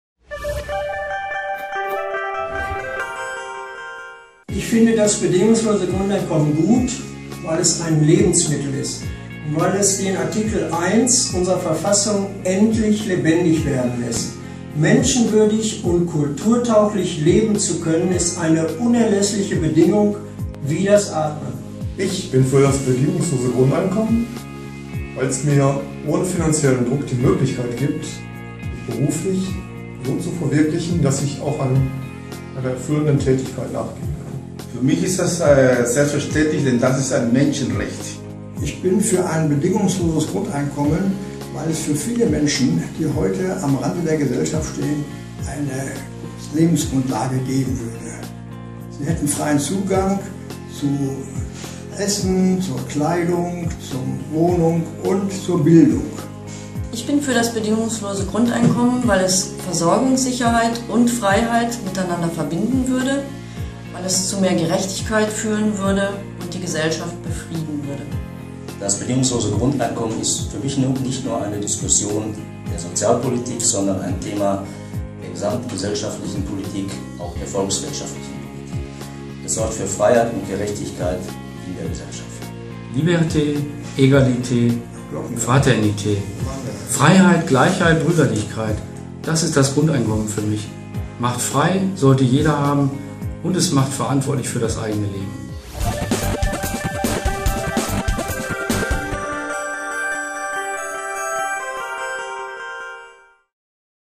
BGE- Meinungen aus Recklinghausen (Grundeinkommen)
Das Bedingungslose Grundeinkommen (BGE) ist in aller Munde. Wir bringen zu dem Thema interessante Gesprächspartner zusammen und laden ein zur Podiumsdiskussion "Bedingungsloses Grundeinkommen" am 1. März 2016, in der Villa Horion, Johannes Rau Platz, 40213 Düsseldorf